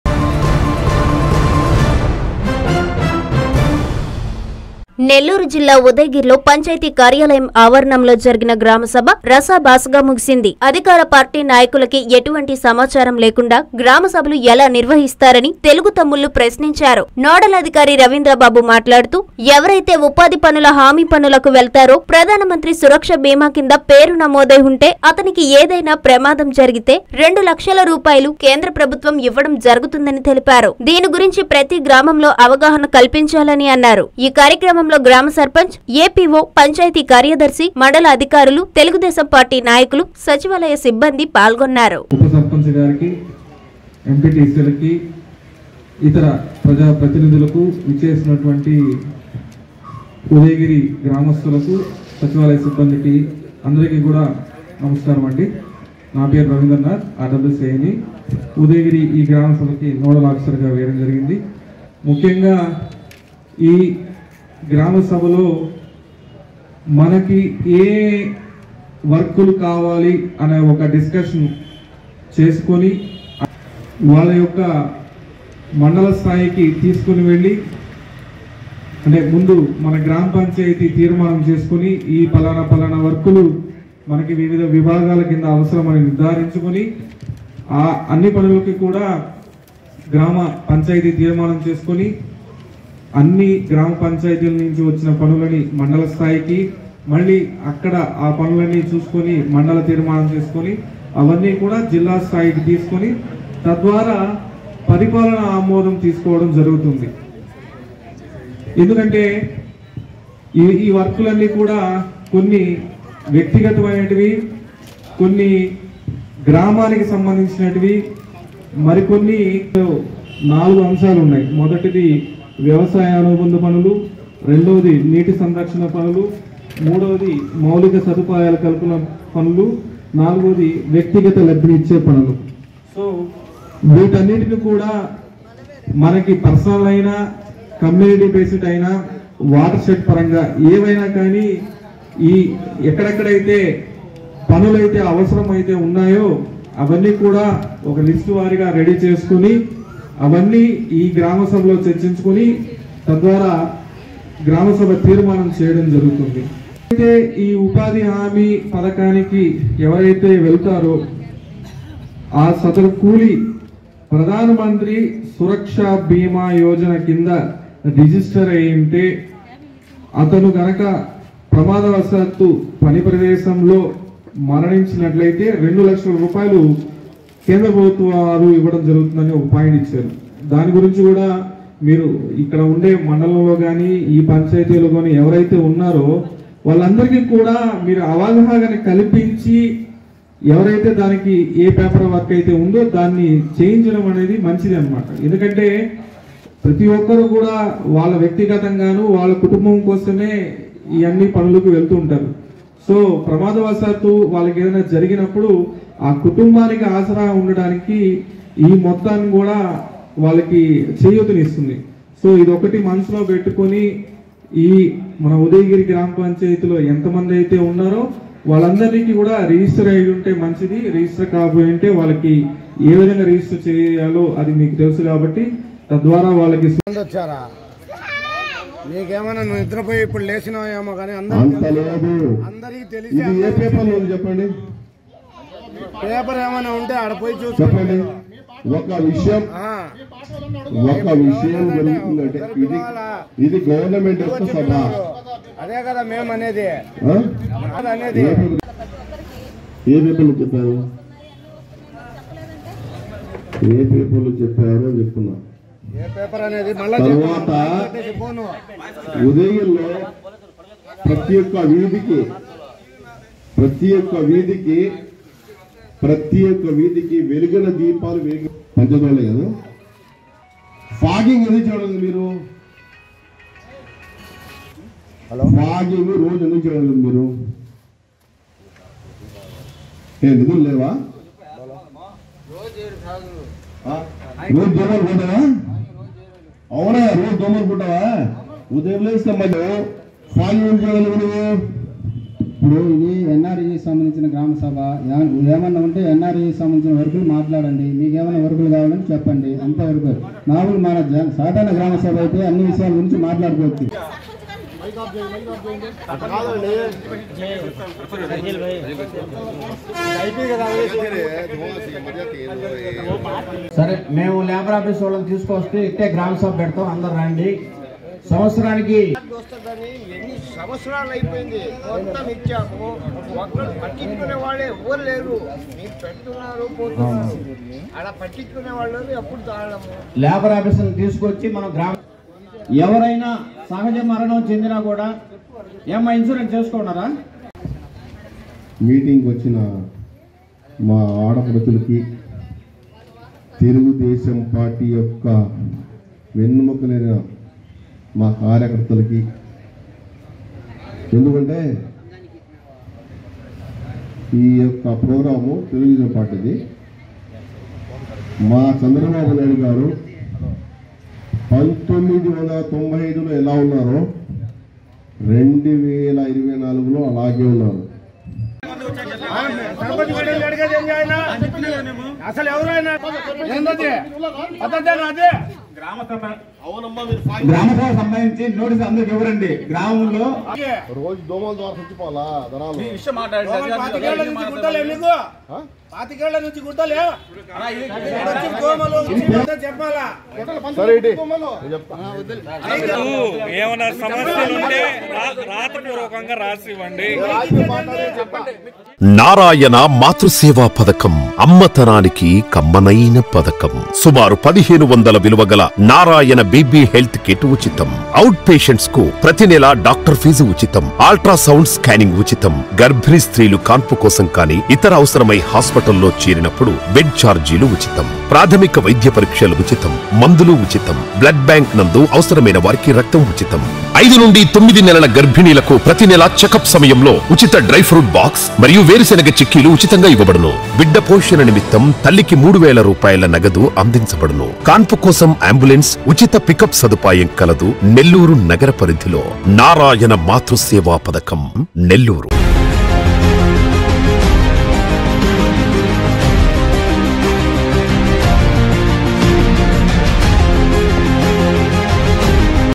ఇంటిమేష‌న్ లేకుండా గ్రామ‌స‌భ‌లా...?
- రసాభాసగా ముగిసిన ఉదయగిరి గ్రామసభ..!